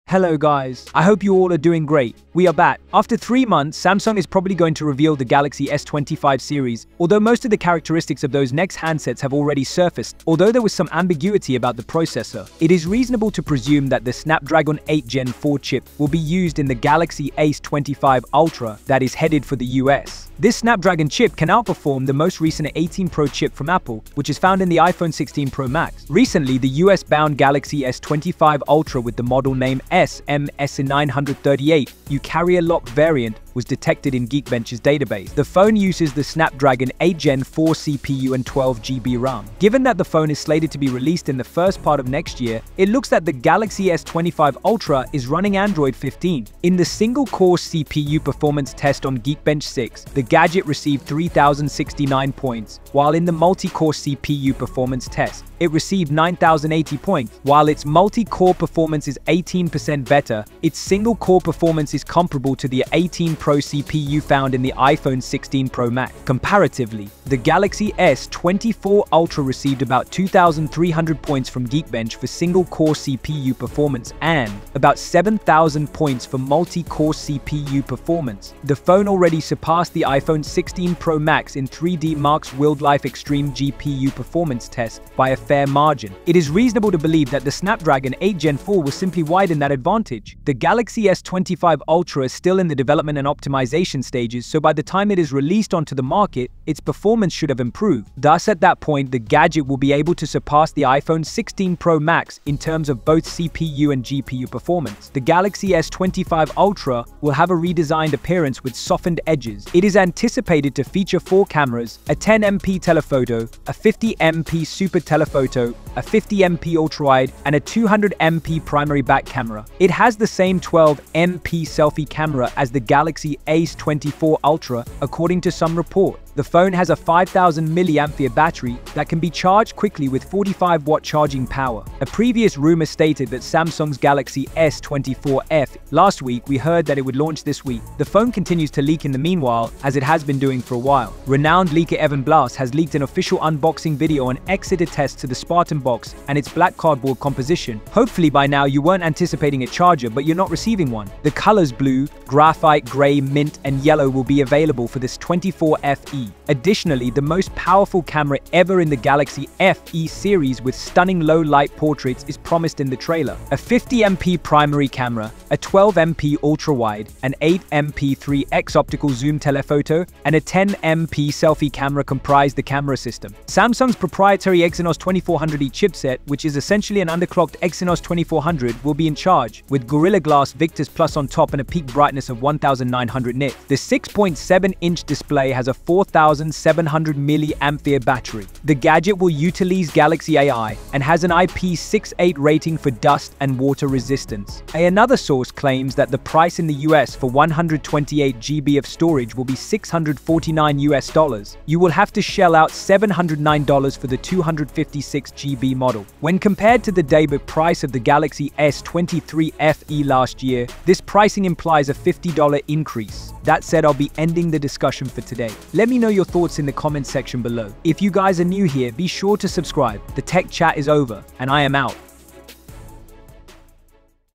Samsung Galaxy S25 Ultra – Better Than Apple!
Hello guys,
After three months, Samsung is probably going to reveal the Galaxy S25 series, although most of the characteristics of those next handsets have already surfaced. Although there was some ambiguity about the processor, it is reasonable to presume that the Snapdragon 8 Gen 4 chip will be used in the Galaxy S25 Ultra that is headed for the US.

This Snapdragon chip can outperform the most recent A18 Pro chip from Apple, which is found in the iPhone 16 Pro Max.
Recently, the US-bound Galaxy S25 Ultra with the model name SM-S938U carrier-locked variant was detected in Geekbench's database. The phone uses the Snapdragon 8 Gen 4 CPU and 12GB RAM. Given that the phone is slated to be released in the first part of next year, it looks that the Galaxy S25 Ultra is running Android 15.